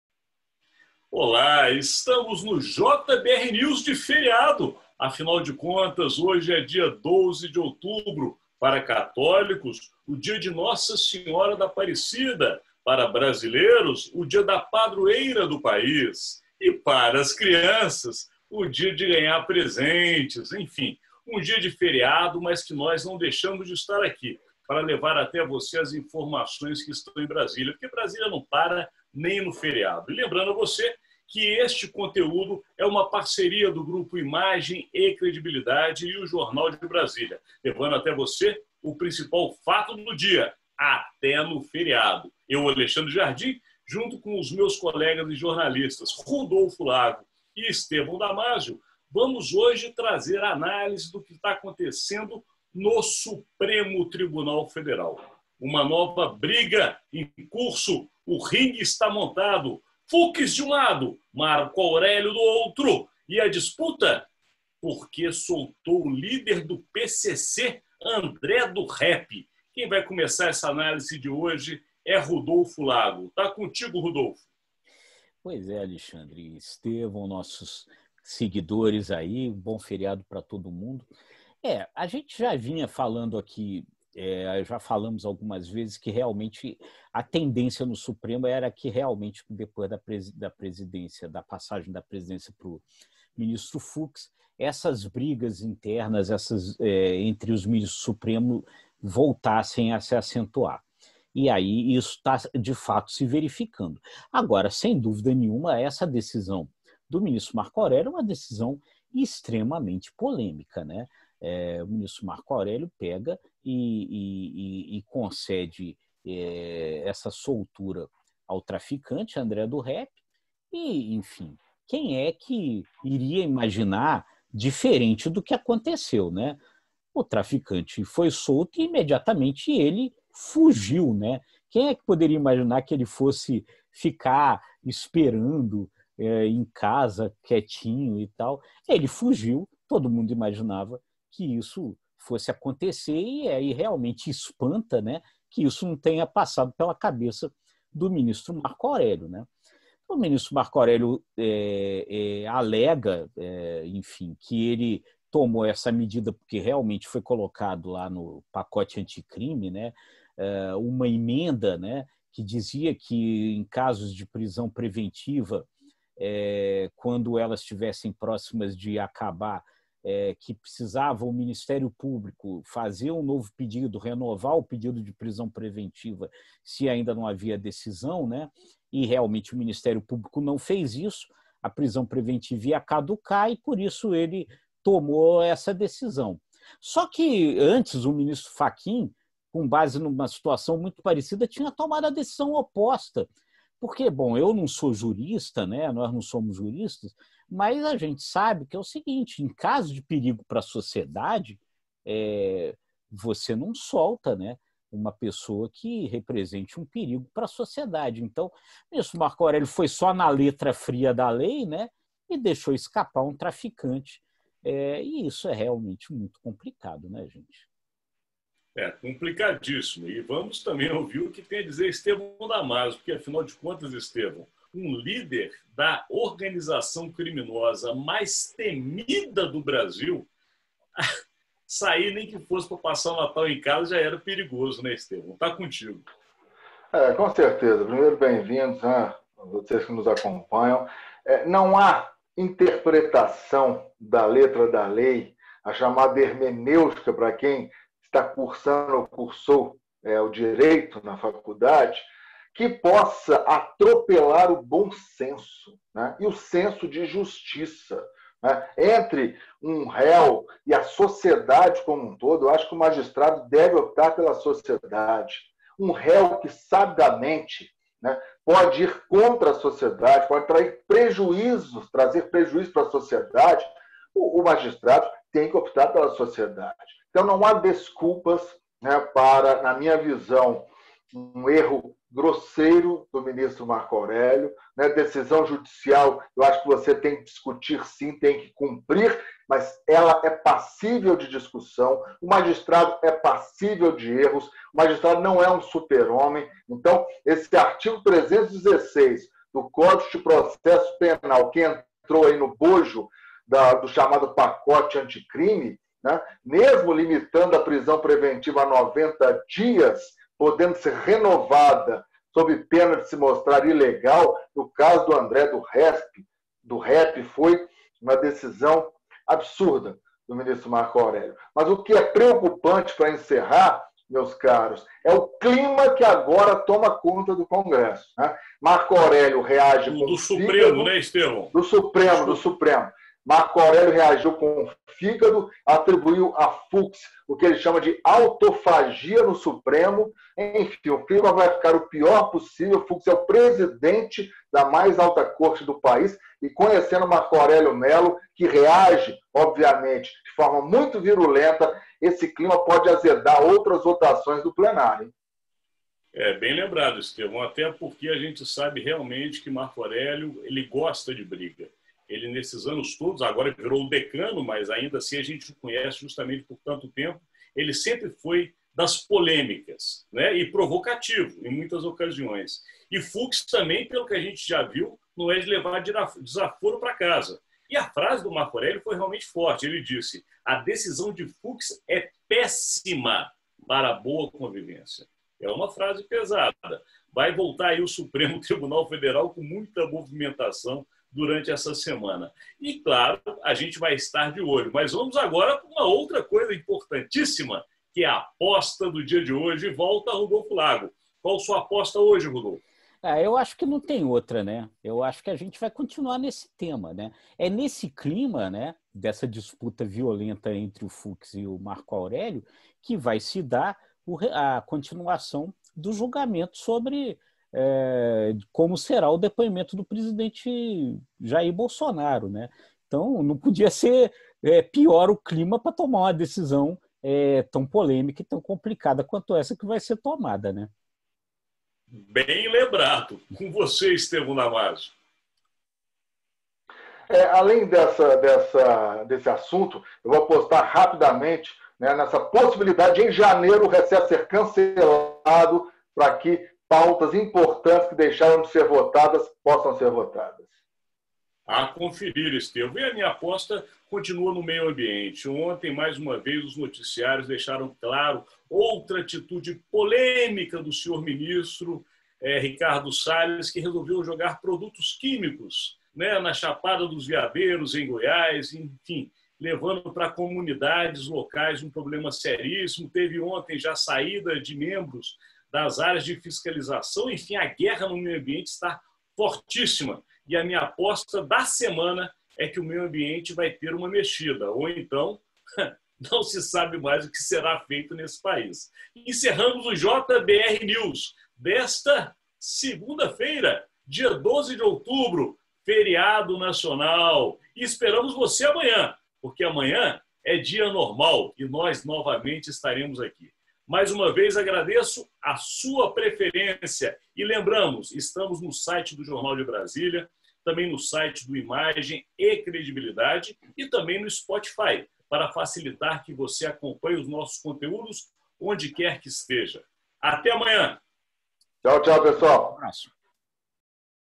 Imagem & Credibilidade Podfast 080: O STF é lugar de fazer Rap?
O ministro Marco Aurélio Mello tomou uma decisão no mínimo polêmica. Emenda adicionada ao pacote anticrime prevê que prisões preventivas caducam em 90 dias se não forem renovadas pelo Ministério Público. Os advogados do traficante André do Rap perceberam a brecha e pediram, assim, a sua libertação. Marco Aurélio a concedeu, e não deu outra: o traficante fugiu. Marco Aurélio poderia ter decidido de outra forma? E a revisão da decisão de Marco Aurélio pelo presidente do Supremo, Luiz Fux, será mais gasolina ainda colocada nessa fogueira? A semana começa quente na alta Corte da Justiça. E esse é o tema do JBrNews de hoje, o podfast Imagem&Credibilidade. Com Alexandre Jardim, Estevão Damázio e Rudolfo Lago.